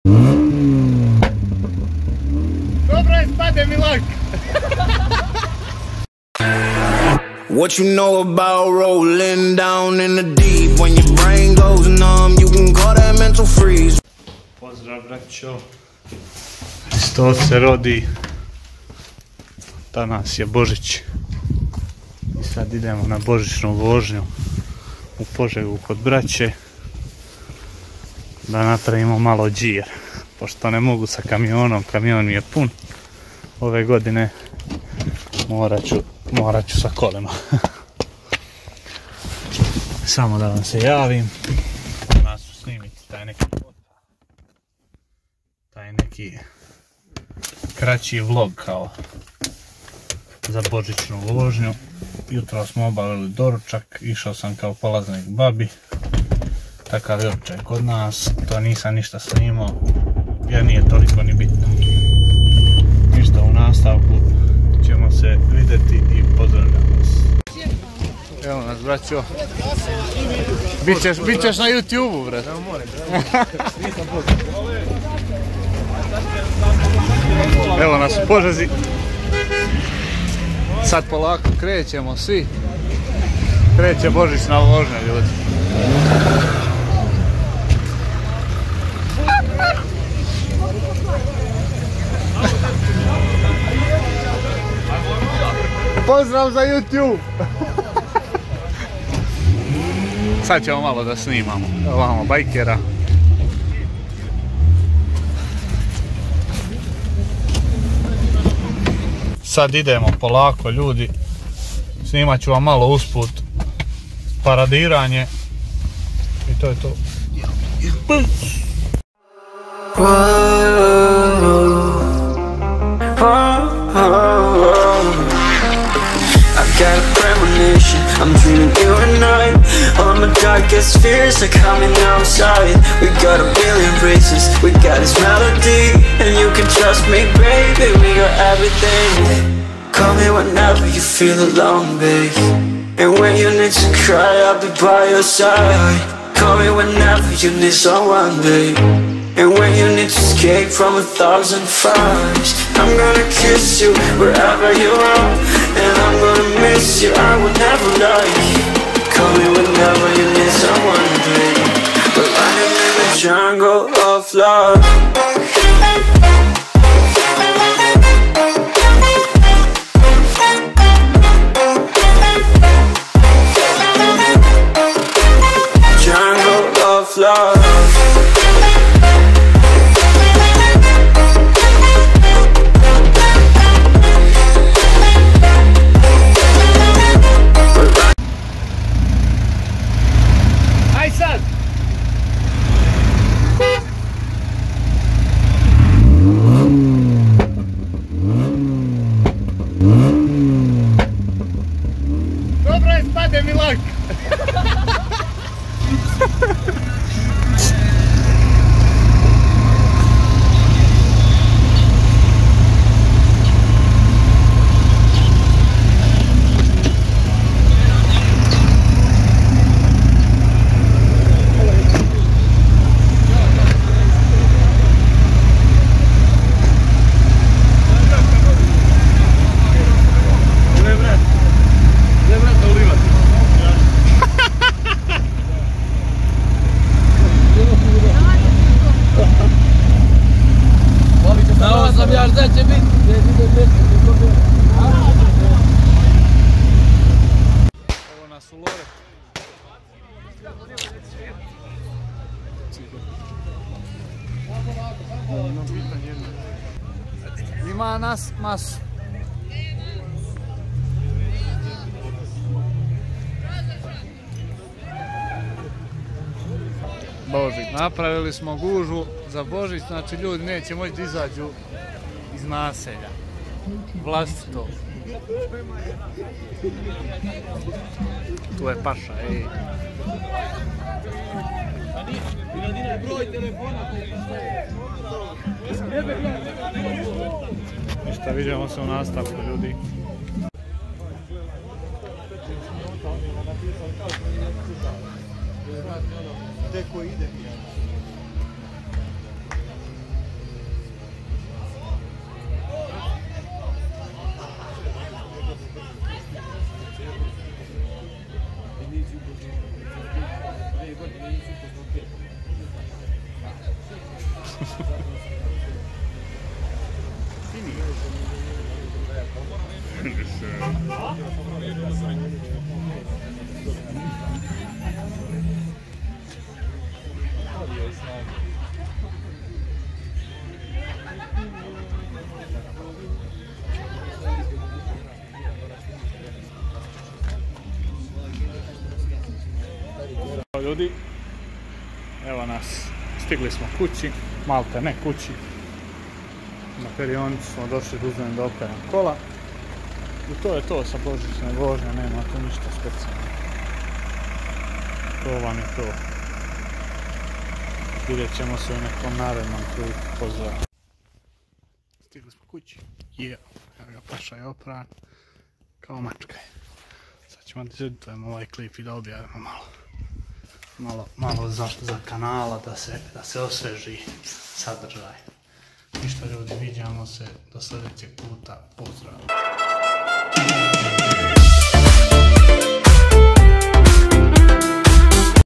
Bozicni vlog, Hristos se rodi ♥
Danas smo iskoristili dan za druzenje sa bracom iz Pozege isli smo na voznju u Loret obliznje selo Pozege, bilo je ekstra, vidimo se i sledece godine u ime boga ♥♥